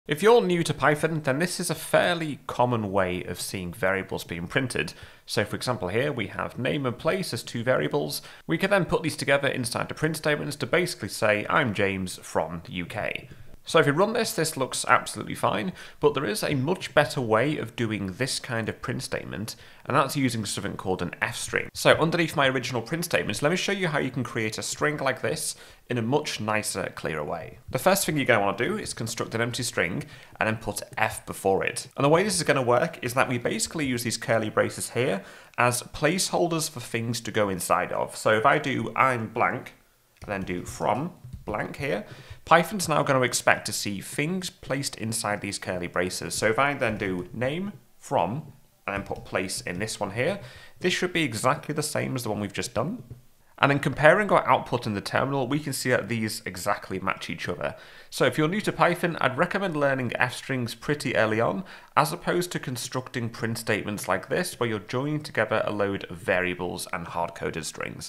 Python 3 | I F-Strings are Easy and Powerful, Here's How! coding
Here's the very basics of how to use a f-string